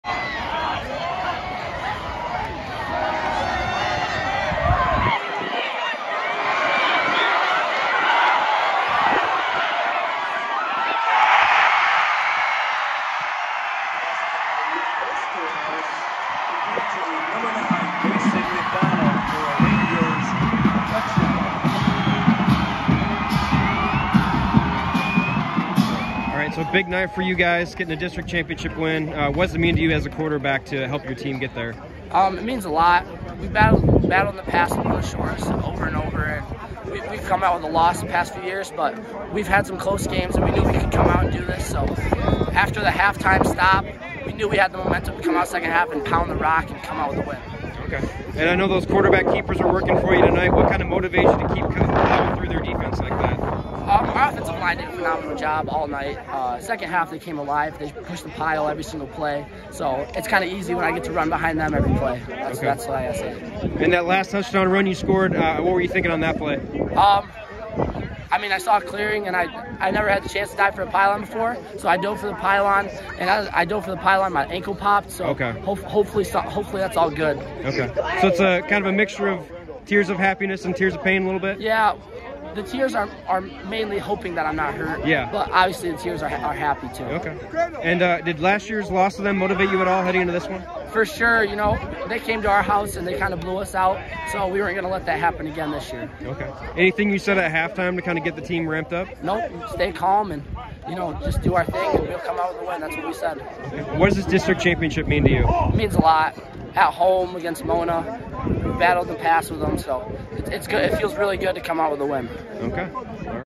Forest Hills Central senior quarterback Justin Osterhouse after district title
Forest Hills Central senior quarterback Justin Osterhouse talks about his team’s 33-21 win over Mona Shores in the 2022 D2 district championship game.